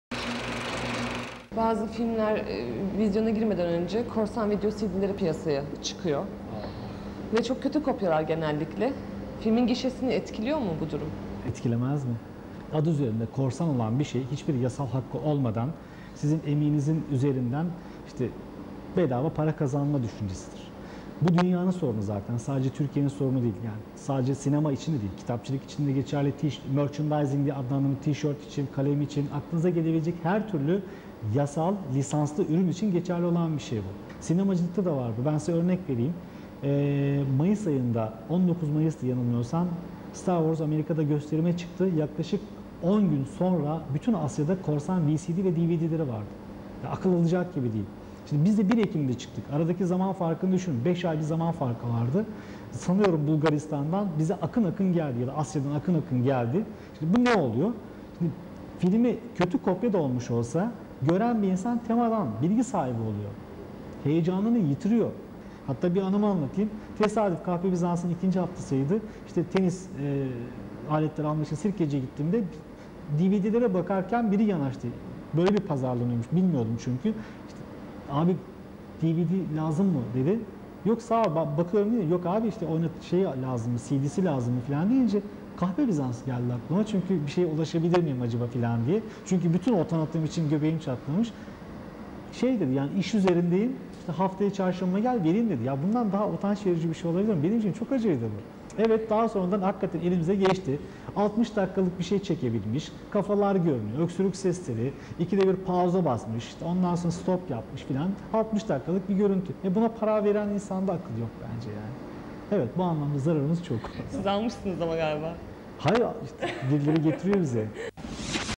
KORSAN, GİŞEYİ ETKİLER Mİ? TÜRKİYE SİNEMASI- SALONLAR- GİŞE ÜZERİNE - 5
Korsan gösterimler günümüzde hem boyut hem de araç değiştirdi. Eskiden korsan kasetler varken, daha sonra dvd, vcd oldu.Şimdi ise smart tvlerden ,cep telefonlarından linklere kolayca ulaşmak mümkün.